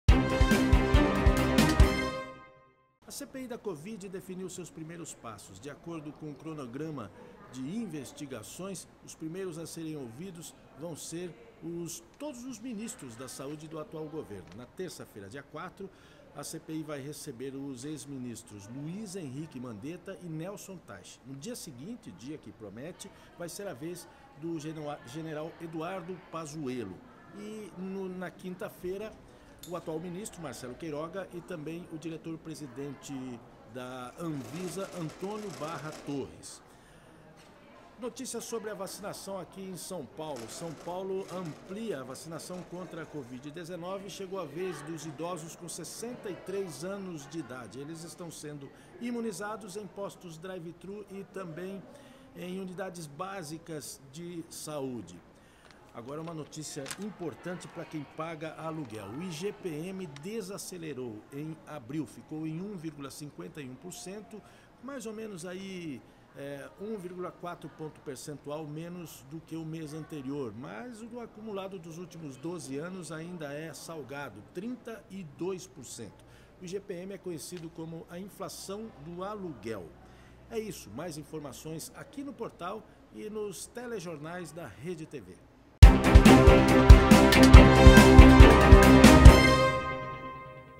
Senadores definem cronograma da primeira semana: ex-ministros e atual convocados
A CPI da Covid definiu seus primeiros passos. De acordo com o cronorama, os primeiros a serem ouvidos serão os ministros da Saúde que passaram pela Pasta nessa pandemia e o atual ministro, Marcelo Queiroga.

São Paulo amplia a faixa etária de vacinação de idosos. Hoje deu início à imunização da primeira dose em pessoas com 63 anos.

Aluguel: o IGPM em abril ficou em 1,4% menor na comparação com março. No acumulado do ano, registra alta de 32%.